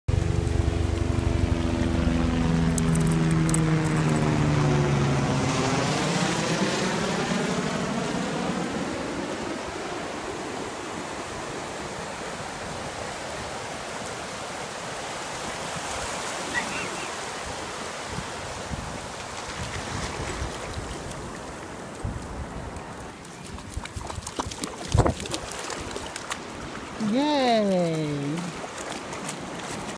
Princess and Moe at the Beach
This is Princess and Moe's first experience with a wave. They ended up playing forever in the water.